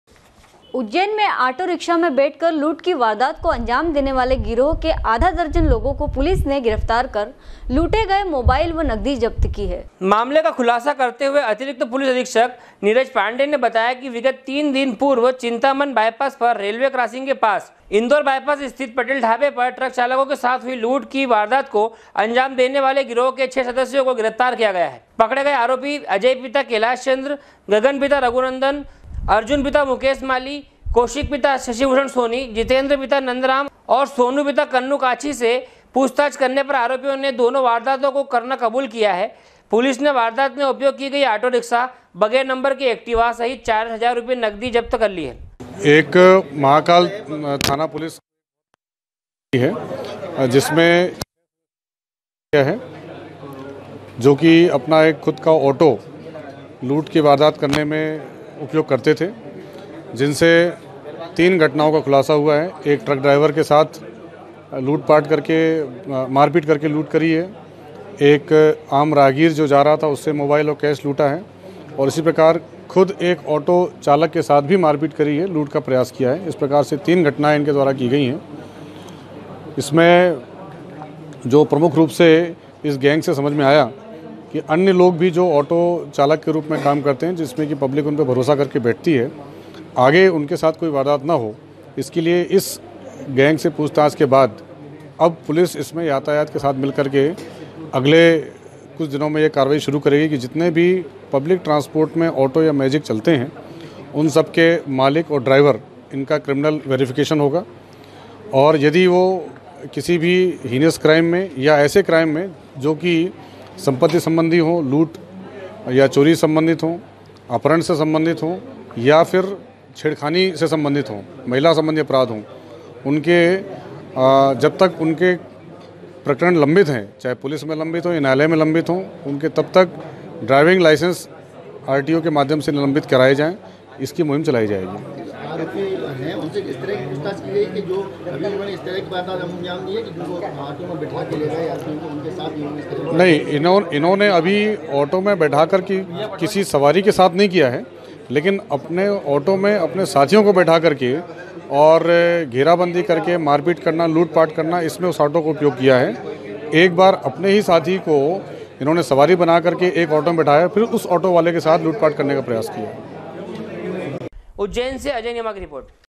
sw24news 18.04.2019 ujjain
उज्जैन में आटो रिक्शा में बैठ कर लूट की वारदात को अंजाम देने वाले गिरोह के आधा दर्जन लोगों को पुलिस  ने गिरफ्तार कर लूटे गए मोबाइल व नगदी जब्त की है।